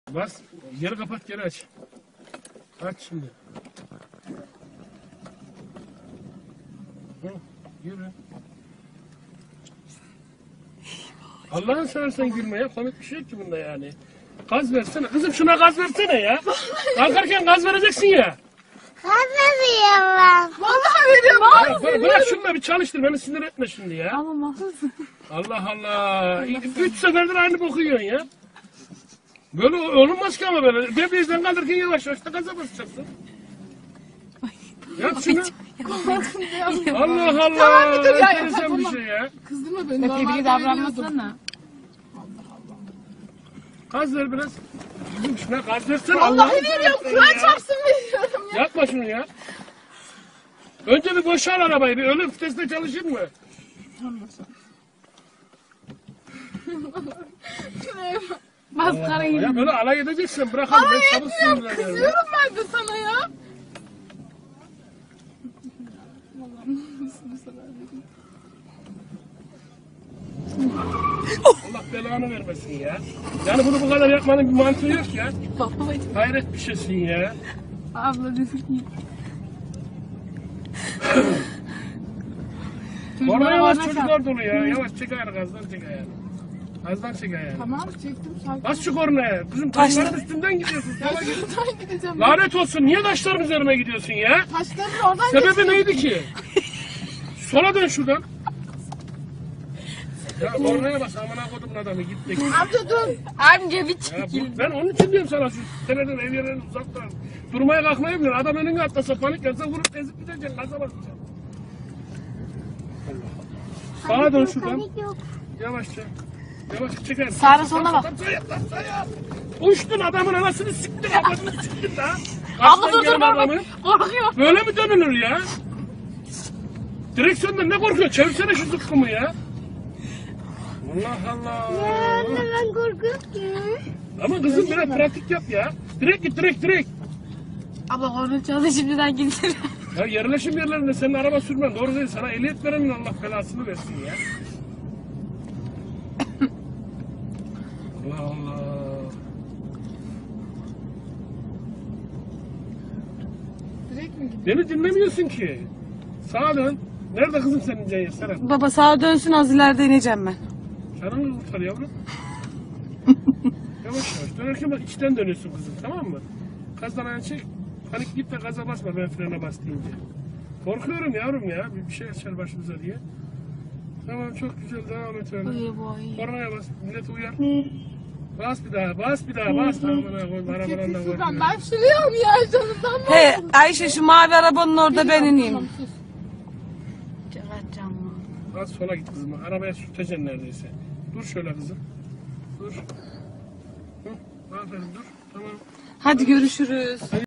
Lanet Olsun Sebebi Neydi Ki !
Kızına araba kullanmayı öğreten babanın dramı.